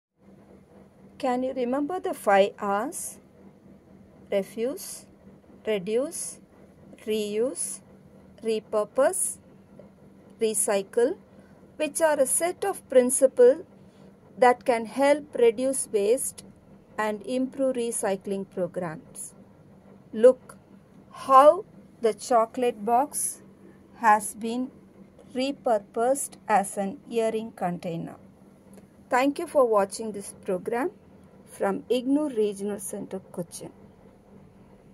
2024 English REPURPOSE Chocolate box into an earring container
English REPURPOSE Chocolate box into an earring container

The 5 R's are a set of principles that can help reduce waste and improve recycling programmes:
1. Refuse: Expressing unwillingness to accept something
2. Reduce: Decreasing the size, amount, or extent of something
3. Reuse: Using something more than once
4. Repurpose: Giving something a new use or purpose
5. Recycle: Recovering material for use by passing

​The video was  captured using the REDMI  mobile handset .

The help rendered by Sh.Muhammed Ansar T.A AEDP, RC Cochin, Sh. R.Chinniah, ASO, REC Kochi and Ms.Reshma Suresh for the recording of this video is acknowledged.